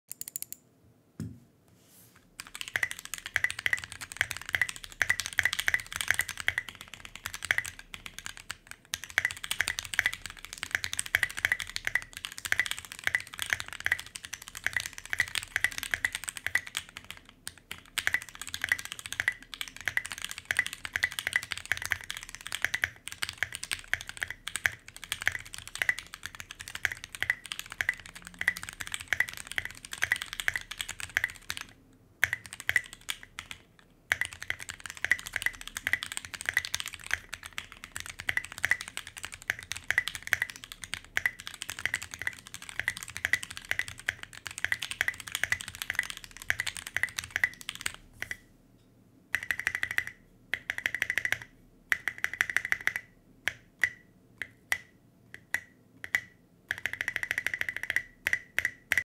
Describe this spacebar sound
Keyboard: Tofu 60% w/ O-ring mod
Plate: Brass
Keycaps: Tai Hao Sakura Michi
Switches: Banana Split (Lubed + filmed)
Stabilizers: C3 Equalz (205g0 & XHT-BDZ)
Artisan: 1 Esc Koi Keycap & 1 Dragon Enter Keycap
Foam: Mekanisk case foam

🔹️Creator code 'Vainsz' (epicpartner)